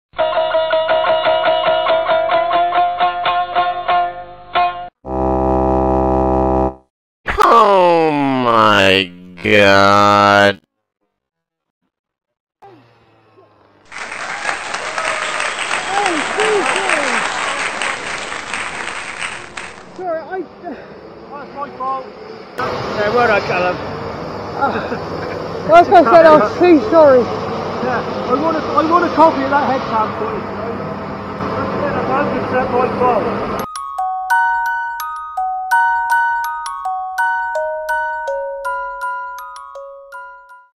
OohMyGod!!!
Simon gets ran over!